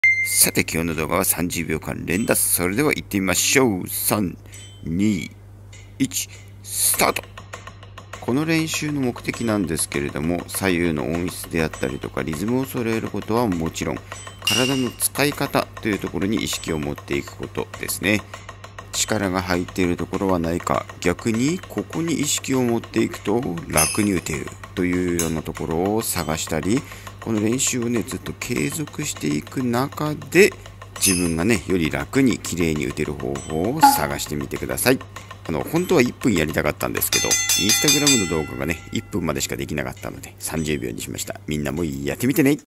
【1分ドラムレッスン】30秒間連打の中で色々な所に意識を向けてみよう
同じフレーズを継続する中で自分の身体の感覚と向き合ってみましょう。「ここに力が入ってるな」とか「肘を引くと楽になるな」など一人一人感じる事はそれぞれ。その中で自分が楽に演奏出来る打ち方を探してみましょう☝️

▼ドラム初心者向けBlog『TOMOドラム塾』はこちら

◎Drums…片平智久(Tomohisa Katahira)

福島県いわき市を中心にドラムレッスンを行っています。